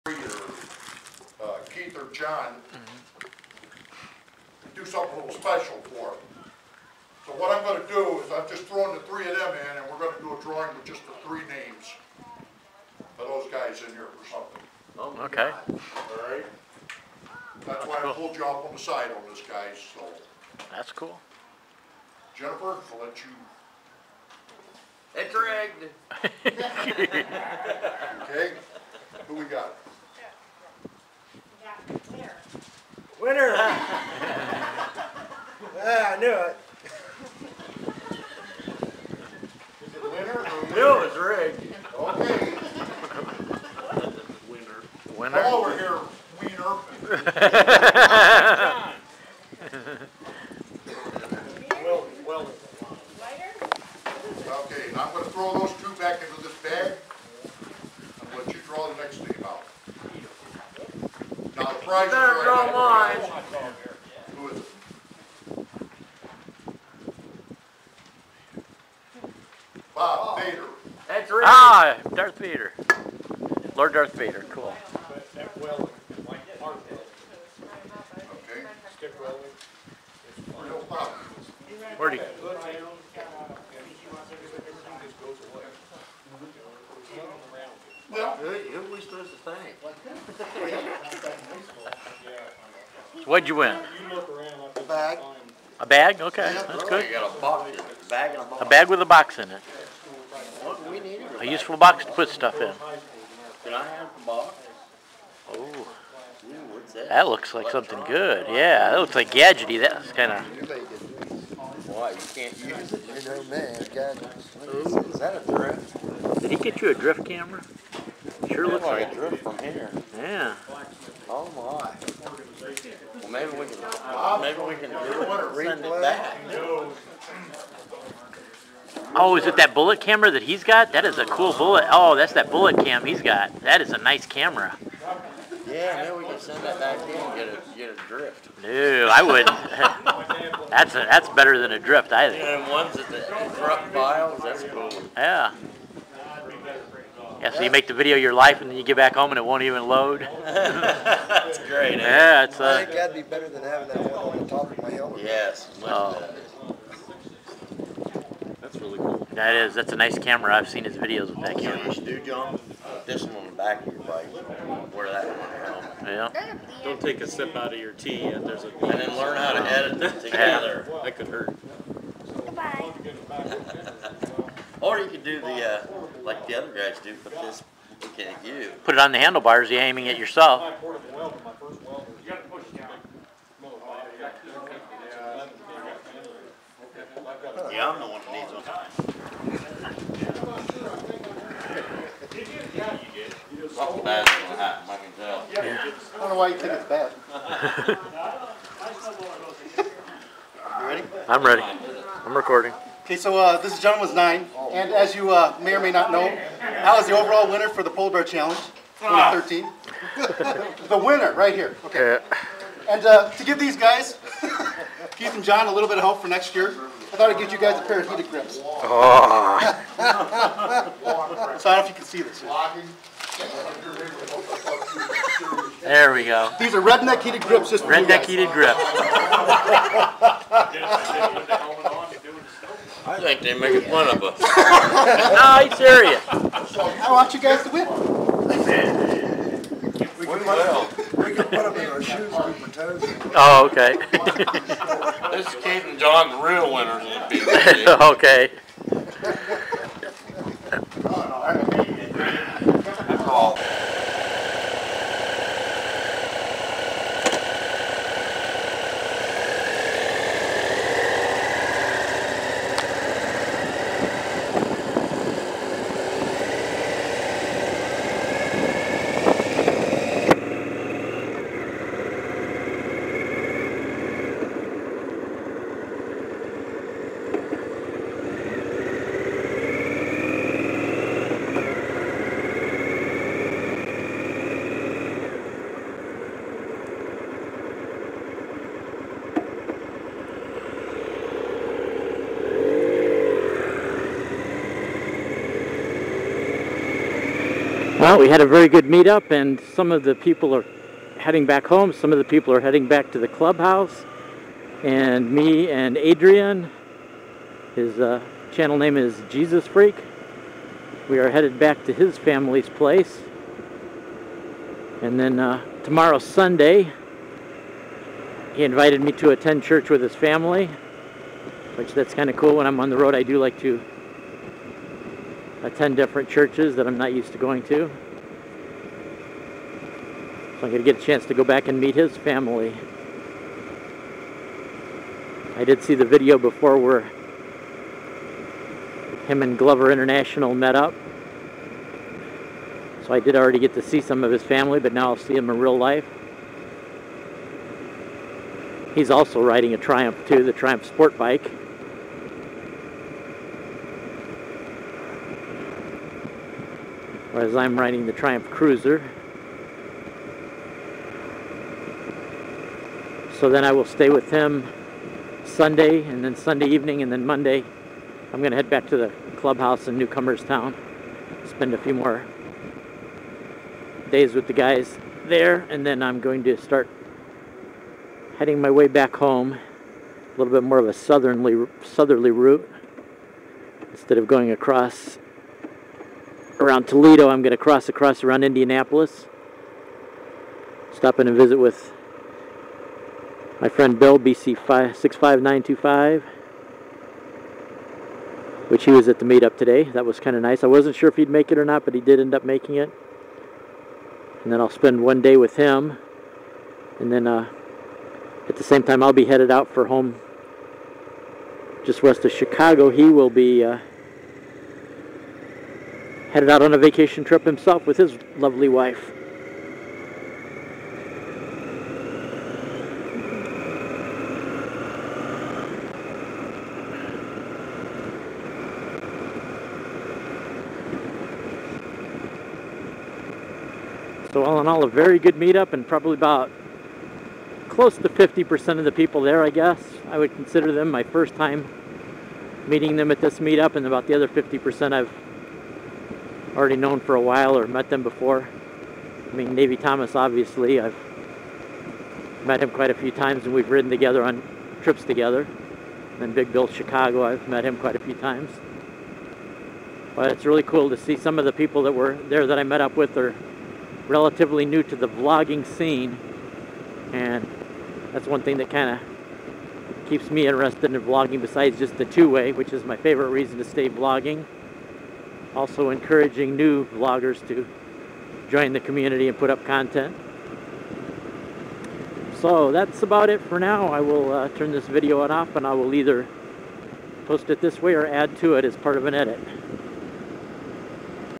August 17th 2013 Meetup Highlights
After the meet I leave out with geesisfreak to spend time with him and his wonderful family.
Then back to the clubhouse to visit some more with the Buckeye Boys and other bros.